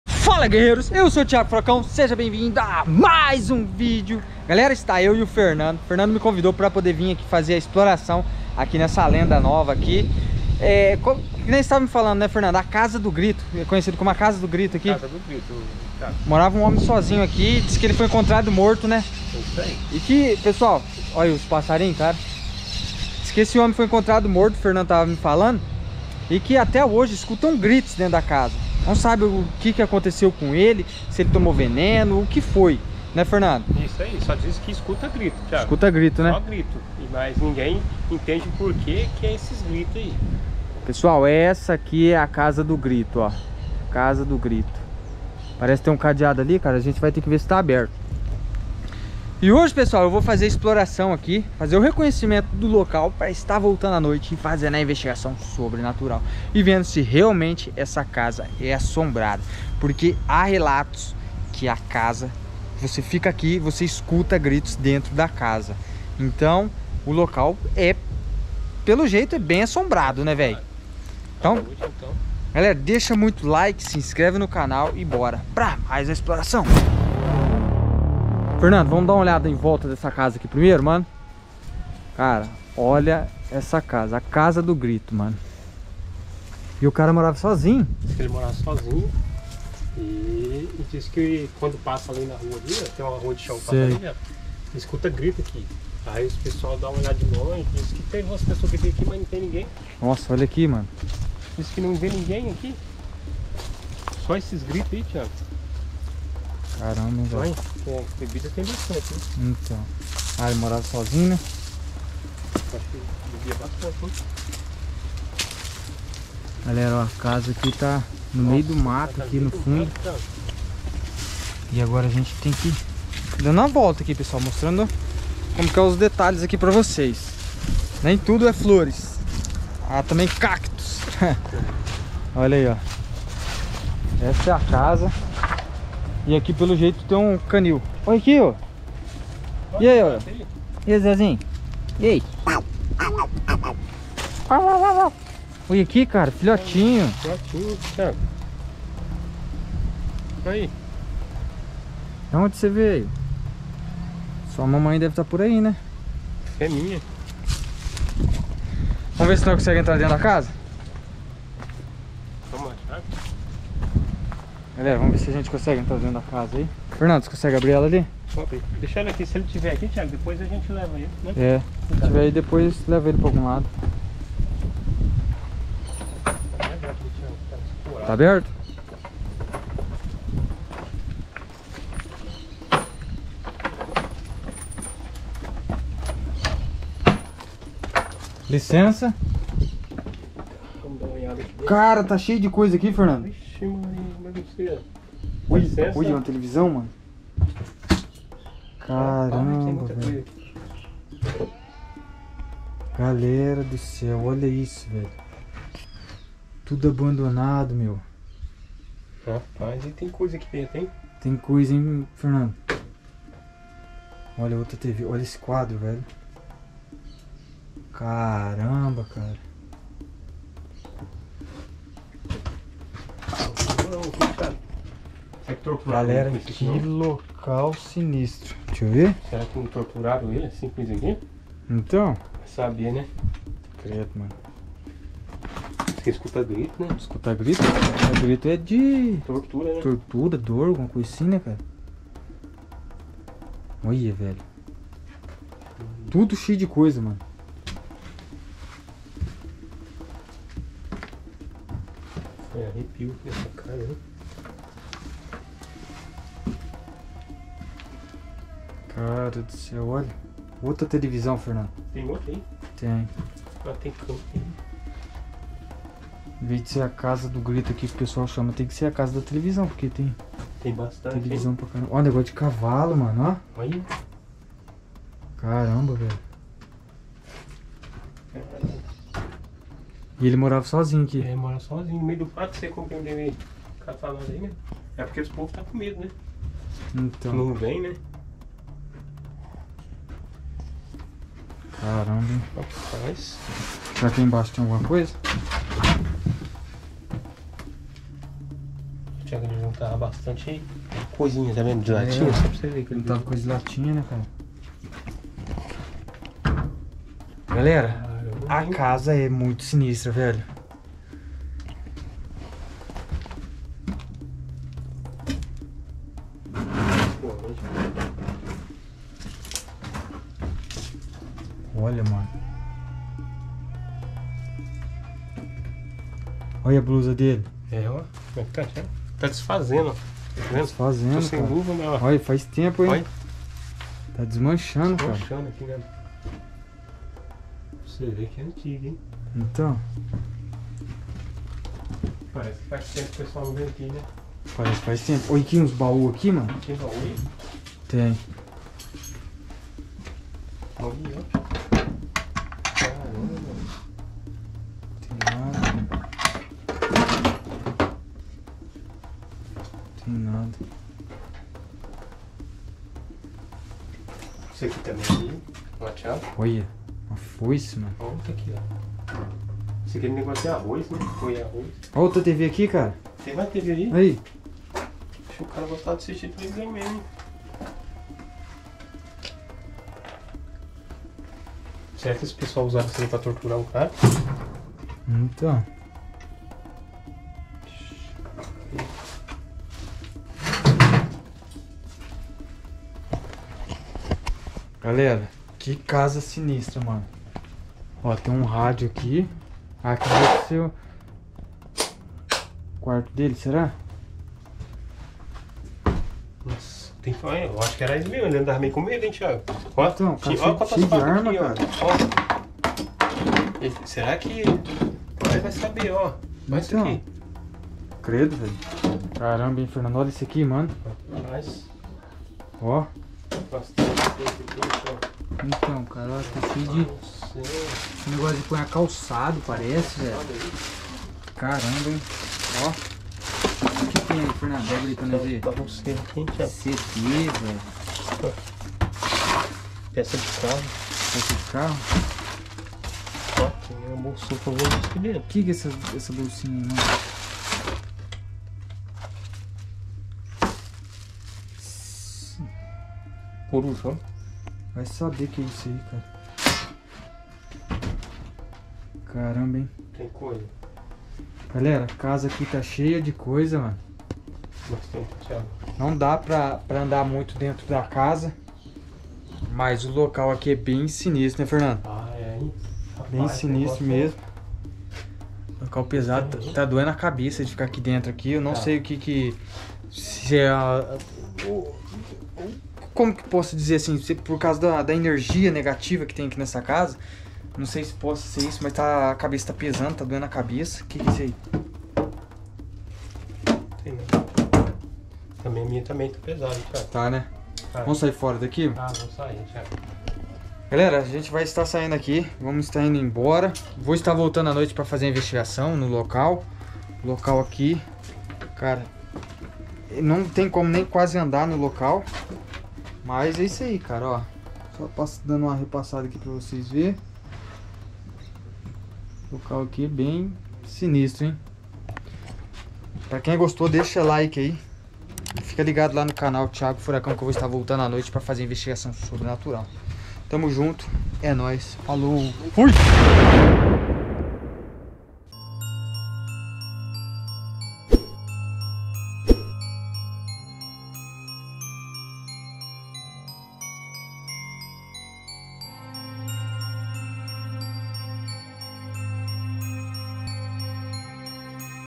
ENTREI NA CASA DO GRITO, IMPRESSIONANTE OQUE TEM DENTRO
ENTREI NA CASA DO GRITO

Maringa -PR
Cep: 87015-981
Thiago Roberto